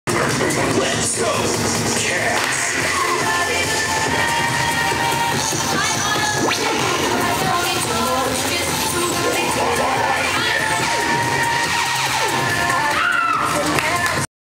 WWMS Cheer Autumn's Quarter Up Scale 360
Autumn's group working on the second stunt sequence for the competition routine.